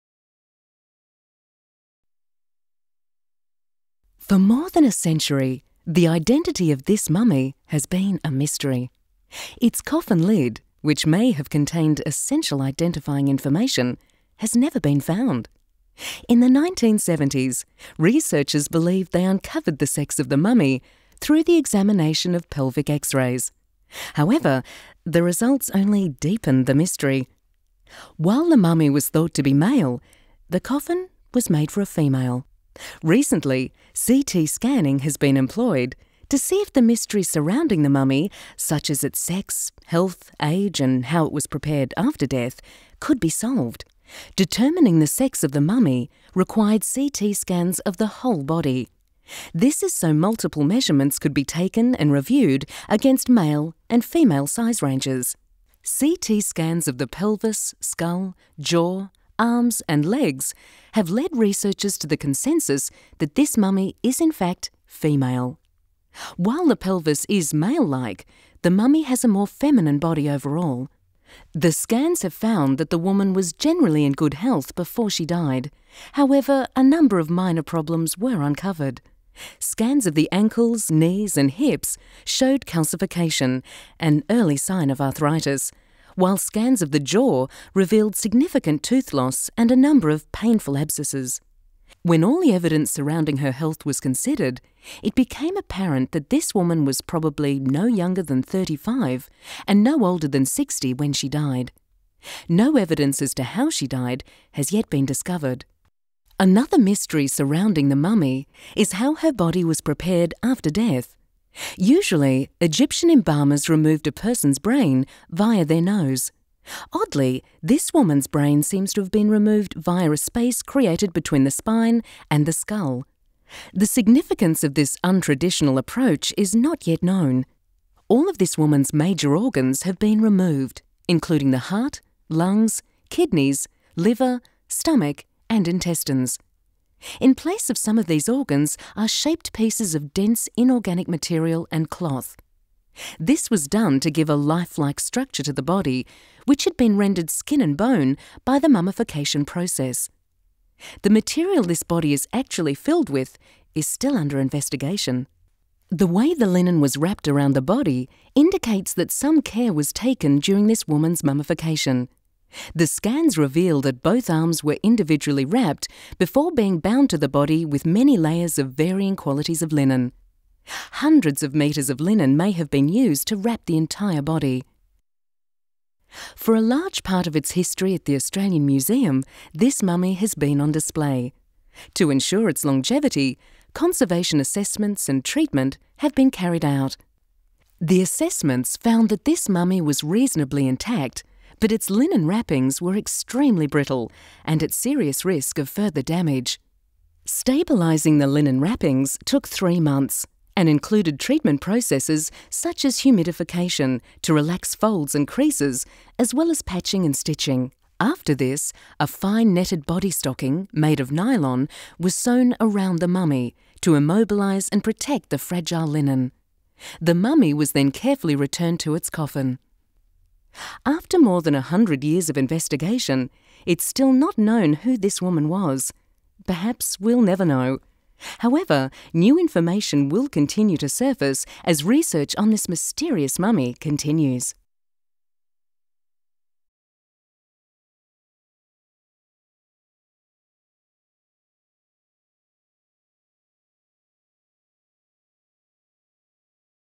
Mummy's Mysteries Unwrapped
Explaining the investigation and conservation of an ancient Egyptian mummy and coffin on display here at the Australian Museum. CT scanning revealed medical details and carbon 14 dating proved it to be older than thought.